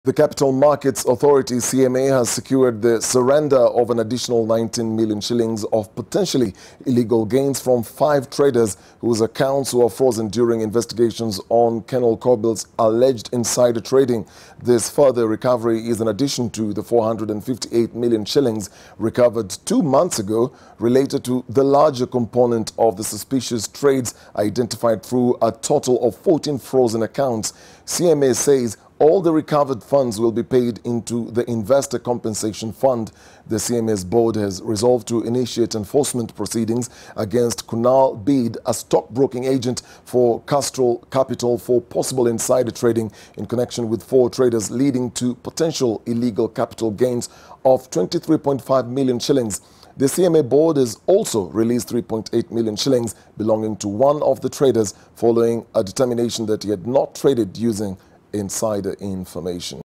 CMA recovers KSh 19M of potential illegal gains in KenolKobil
The Capital Markets Authority, CMA, has secured the surrender of an additional 19 million Shillings of potentially illegal gains from 5 traders whose accounts were frozen during investigations on Kenol Kobil's alleged insider trading.